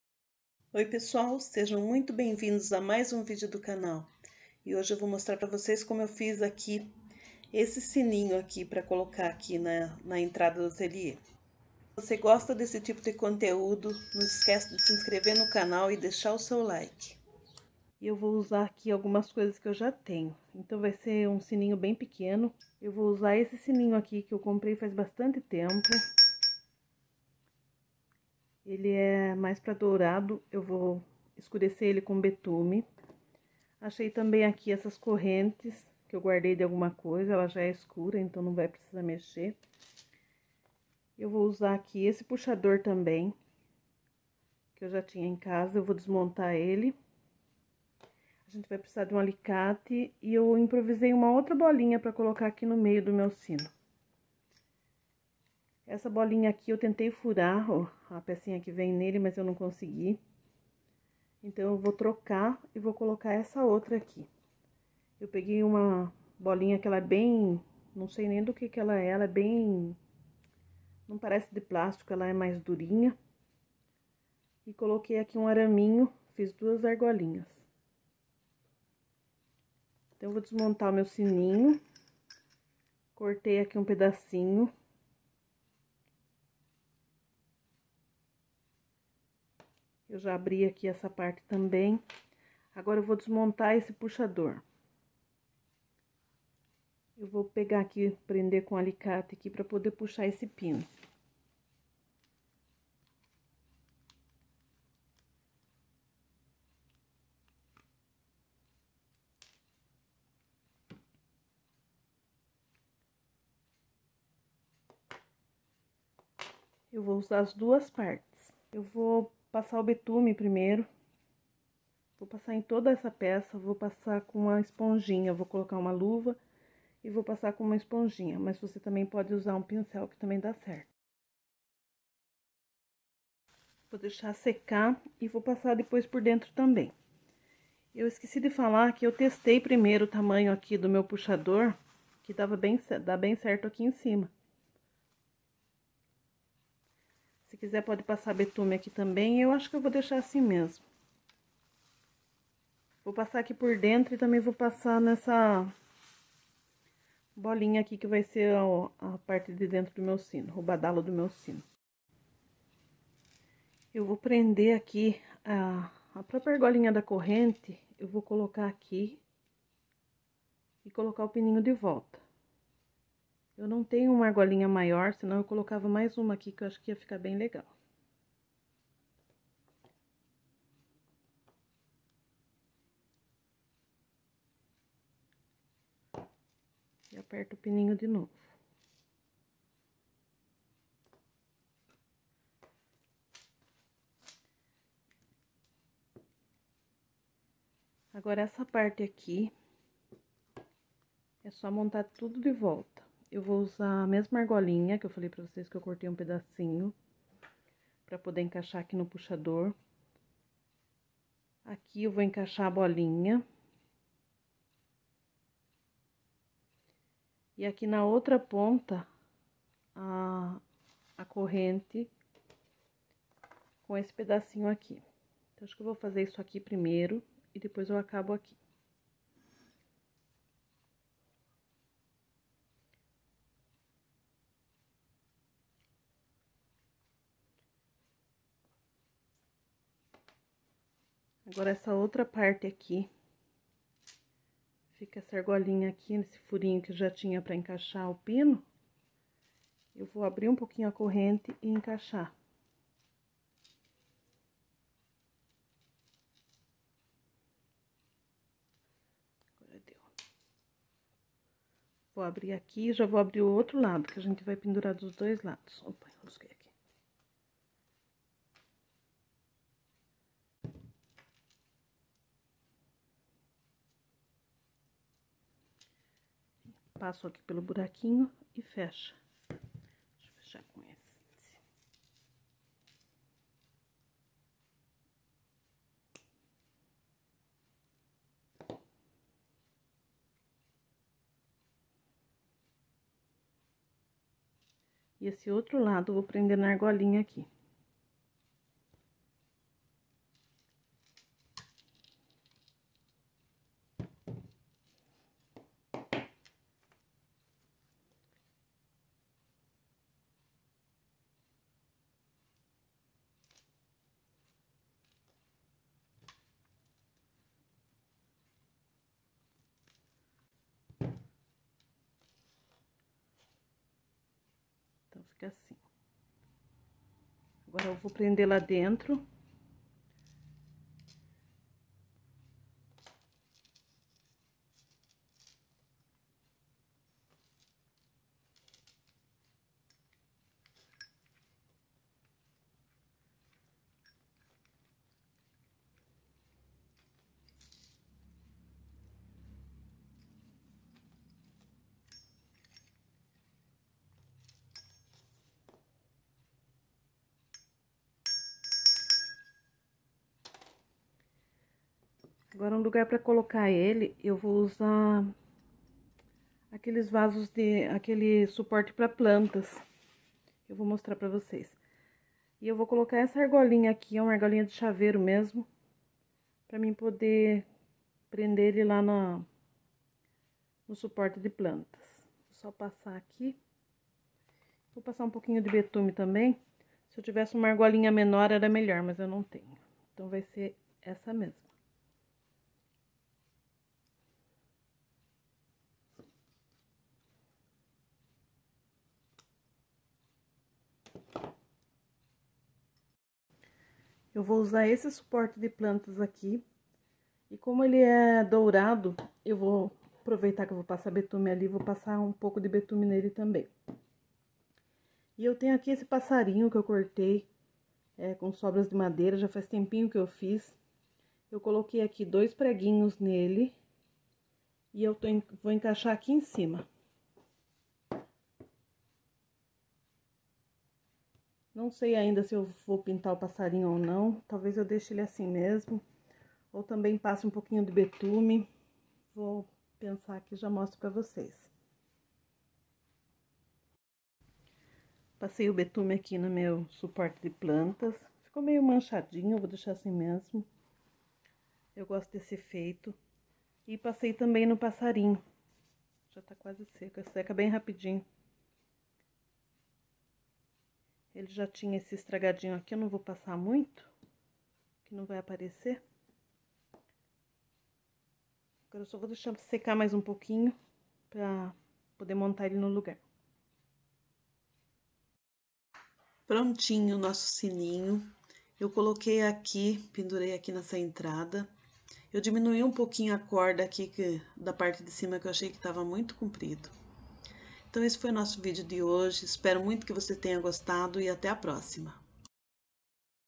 Sininho farmhouse econômico
Estava namorando um sininho desses que coloca na entrada da casa, bem com cara de fazenda. Mas não dá pra sair comprando tudo o que desejo e também por outro lado adoro fazer as coisas pra minha casa. Então saí catando umas coisas por aqui e consegui fazer o meu sininho, talvez vc não tenha as mesmas coisas que eu, mas dá uma olhadinha com carinho para o que vc tem aí que com certeza vc vai conseguir criar peças lindas para sua casa, e ainda por cima ter o gostinho de saber que foi feito por vc.